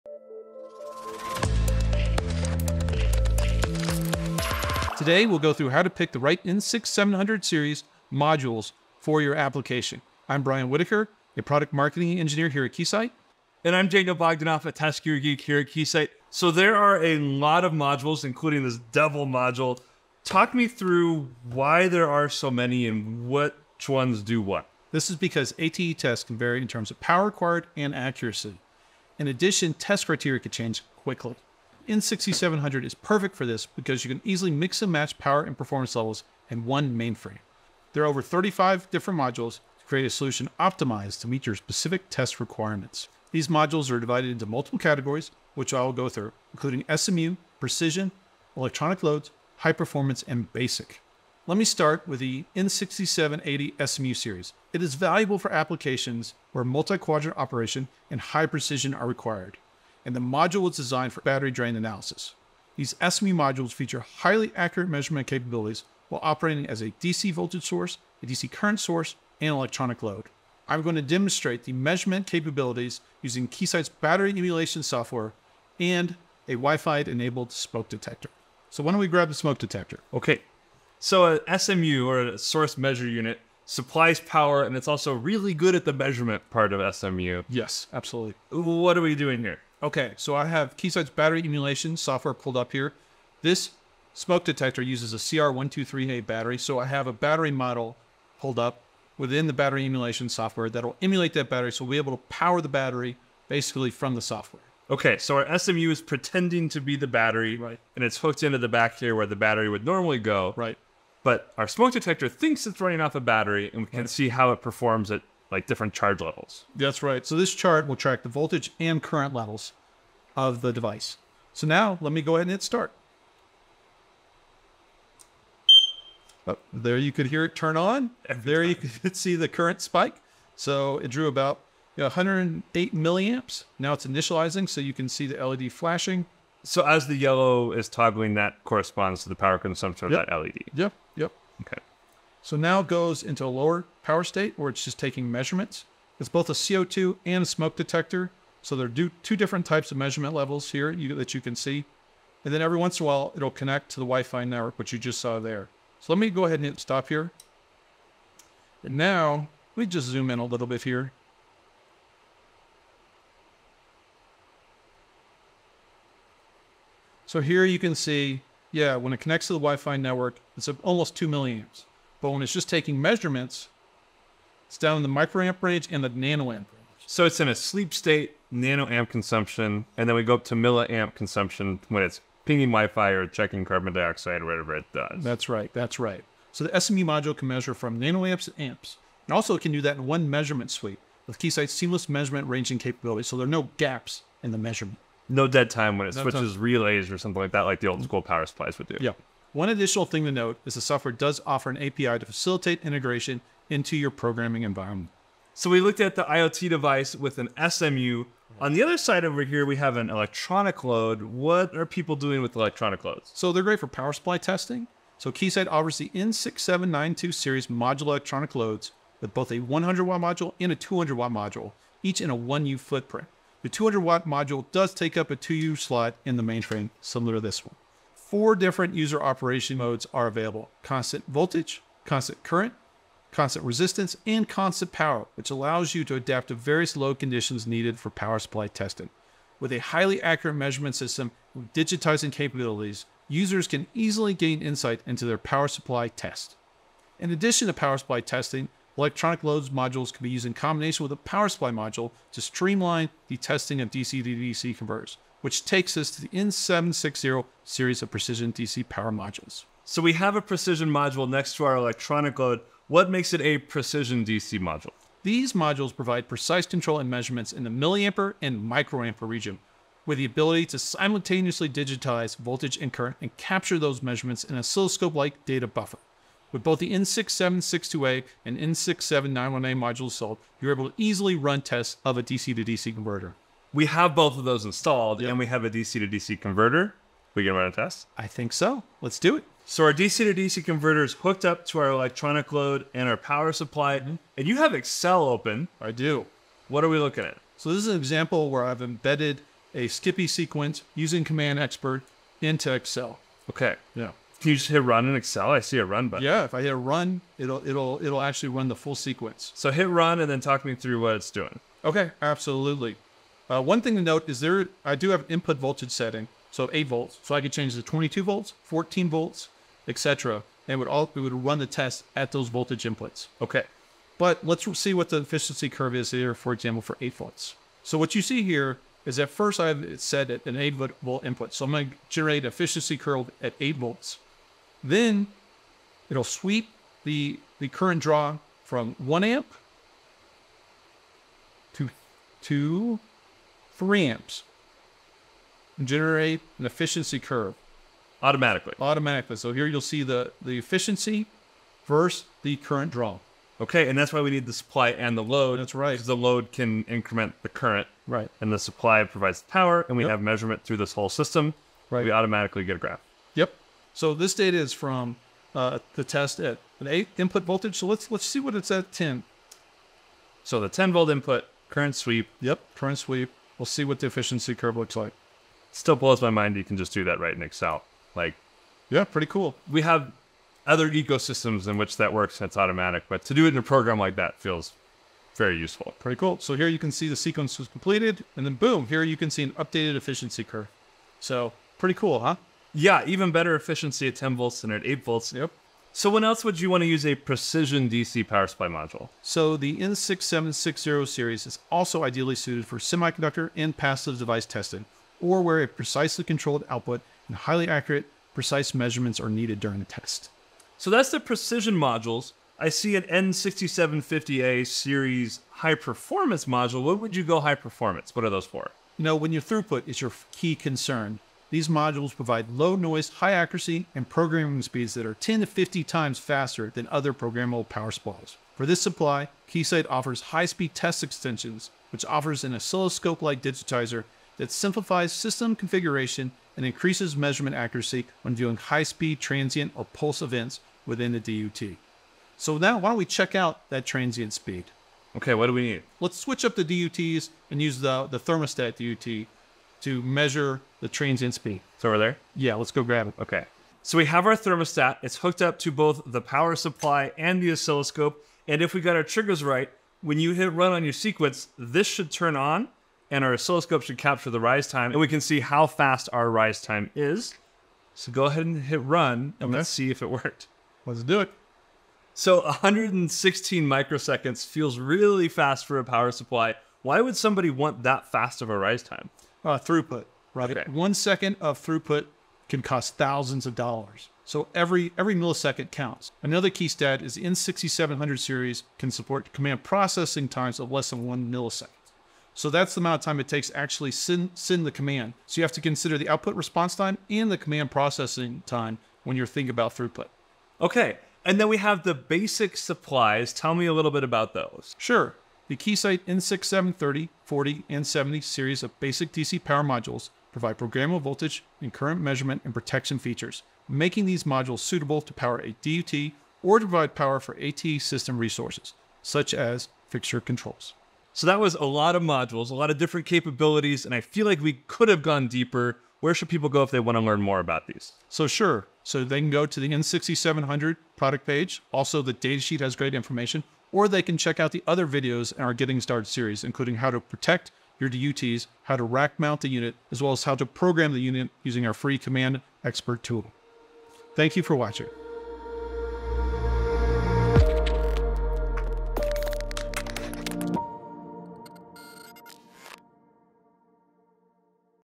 Selecting the Right Modules - N6700 Quick Start Guide Episode 5
Join us and learn how to select the appropriate N6700 modules for your application. Keysight offers over 35 modules at different power and performance levels. In this video, we will discuss and demonstrate the capabilities of various module categories.